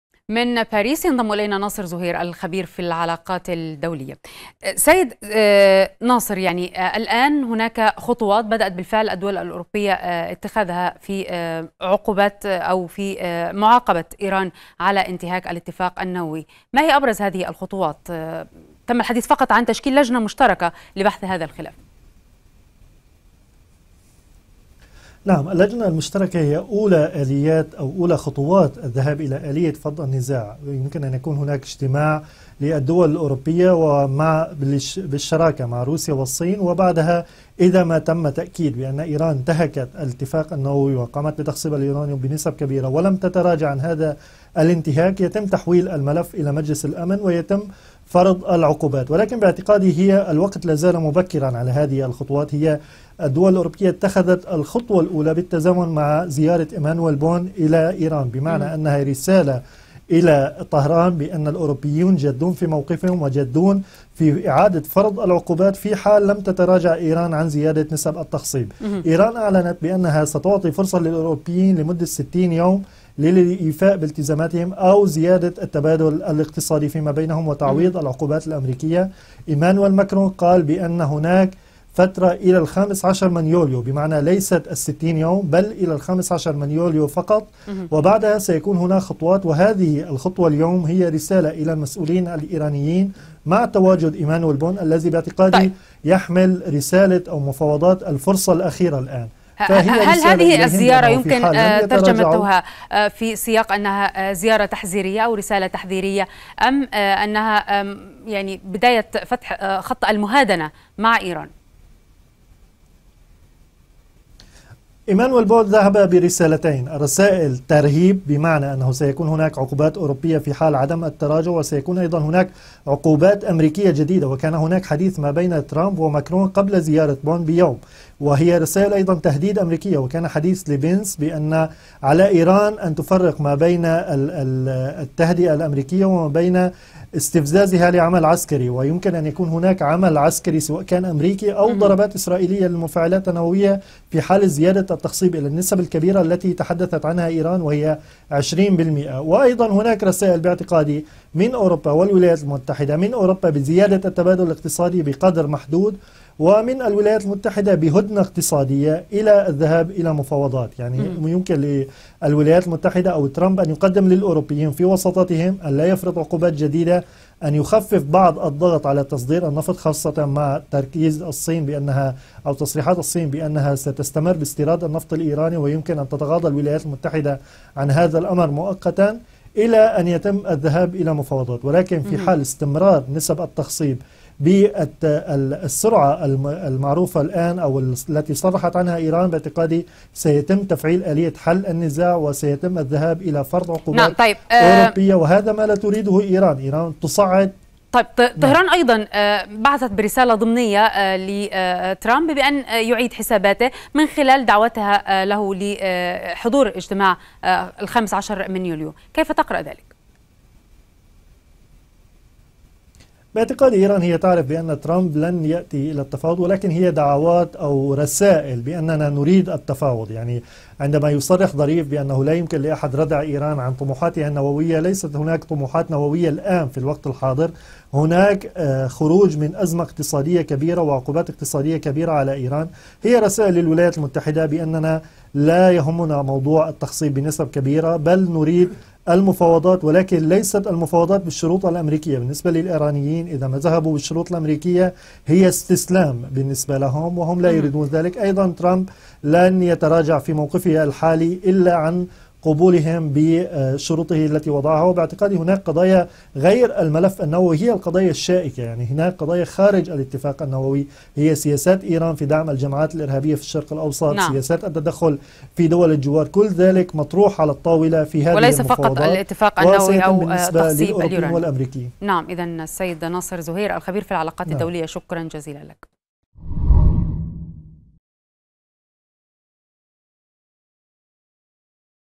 بيان رباعي أوروبي يطالب إيران بوقف خرق الاتفاق النووي .. ما هي السيناريوهات المقبلة؟
اتخذت الدول_الأوروبية أولى خطواتها نحو معاقبة إيران على انتهاك الاتفاق النووي واستندت في ذلك إلى آلية حل الخلافات في الاتفاق. وقال وزراء خارجية بريطانيا و#فرنسا و#ألمانيا بالإضافة إلى مسئولة السياسة الخارجية بالاتحاد الأوروبي في بيان إن إيران تمارس أنشطة لا تتفق مع التزامتها بموجب الاتفاق، مما يستلزم عقد لجنة مشتركة على وجه السرعة لبحث الخلاف الذي قد يؤدي إلى إعادة فرض العقوبات الدولية التي تم رفعها عن إيران بموجب الاتفاق... المزيد من التفاصيل يتحدث من باريس ناصر زهير الخبير في العلاقات الدولية، مع سينار سعيد، عبر الفقرة الإخبارية.